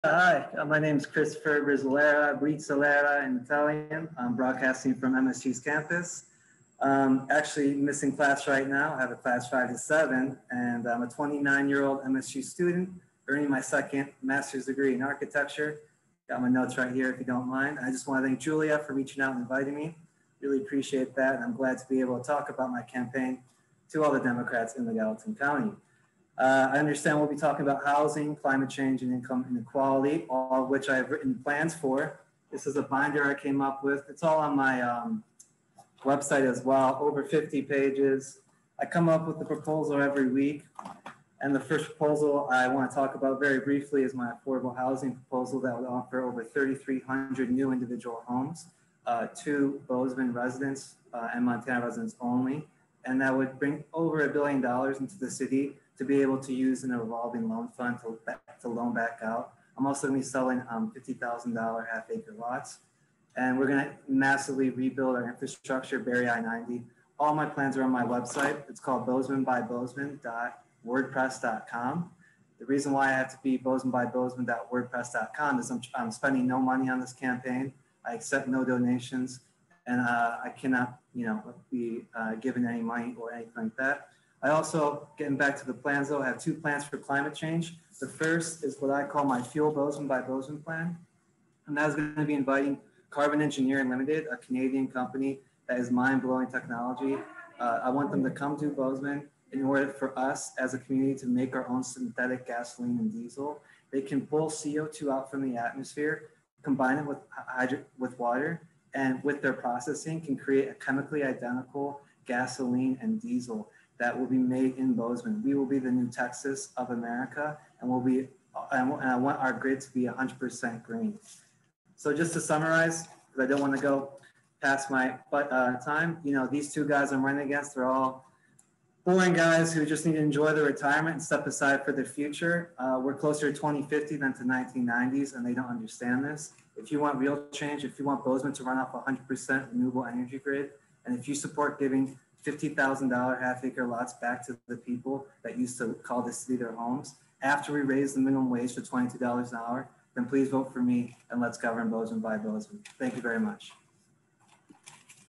Christopher Brizzolara, Bozeman City Mayor Candidate 2021
Your neighbors in the Gallatin County Democrats hosted a community conversation on August 25 with municipal candidates from Belgrade and Bozeman. These positions are up for election on November 2, 2021 and are all nonpartisan races.* At the start of the conversation, each candidate introduced themselves.

Christopher Brizzolara is running for Bozeman City Mayor. This is his introduction.

*Remember, in nonpartisan races, the Gallatin County Democrats do not make endorsements.*

Learn more and get involved at the local level to effect political change